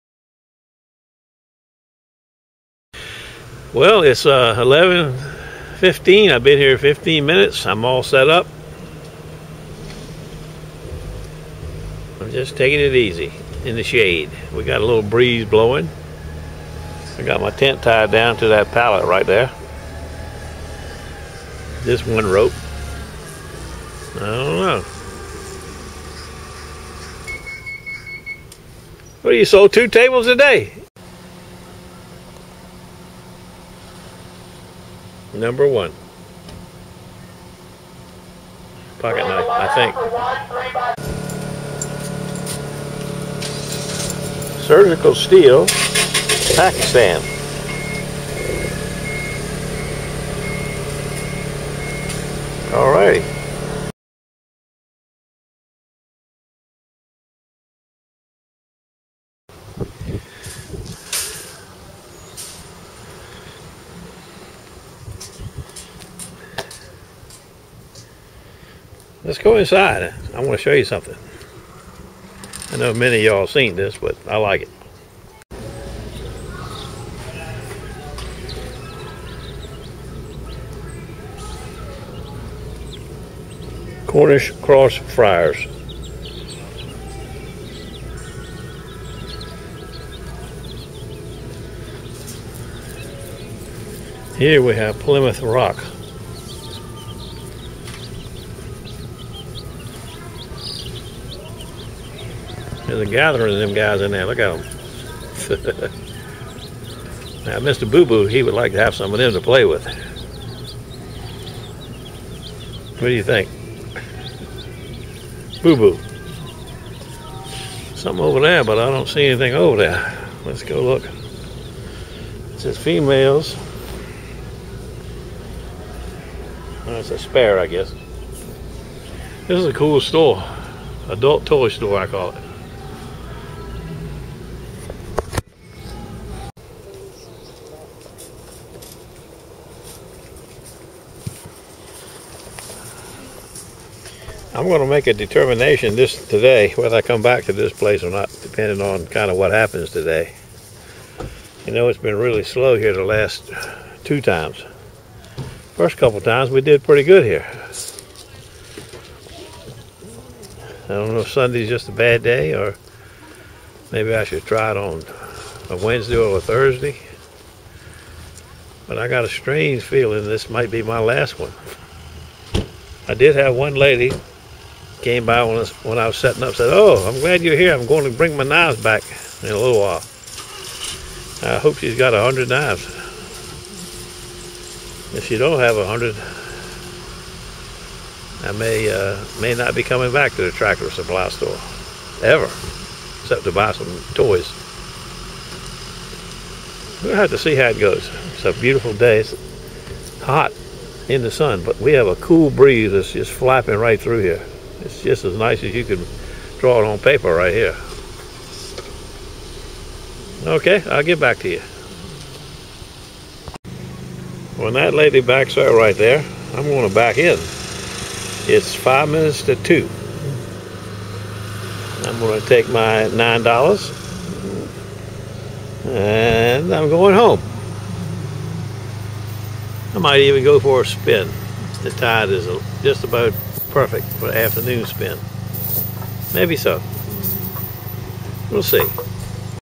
TRACTOR SUPPLY STORE TODAY FOR BLADE SHARPENING
should read 1 blade sharpening. so, it went from 3 blades, to 2 blades and today 1 blade.

i will not return to this location.

Please Visit my Knife Web Page for info.

GEAR I USE:
KNIFE STUFF
1 x 42" Kalamazoo  x 3
1 x 42" Viel
10" Eastwood Buffer - 2 speed
6" Baldor Buffer
3m Trizact sharpening belts